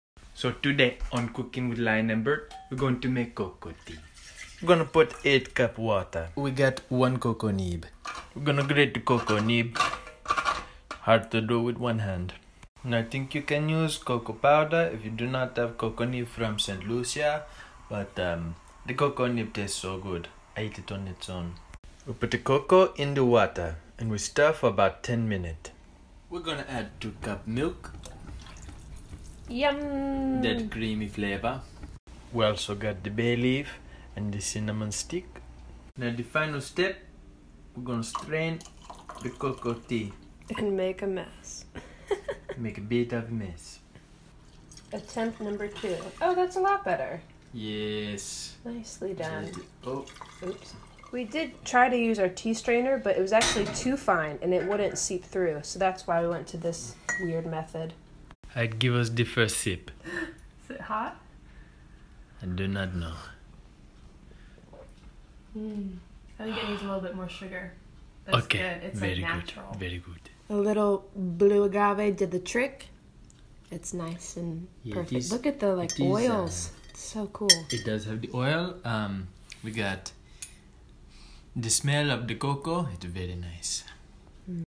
Making Cocoa Tea At Home!
This was our favorite drink from our honeymoon, so we brought a little Paradise gone with us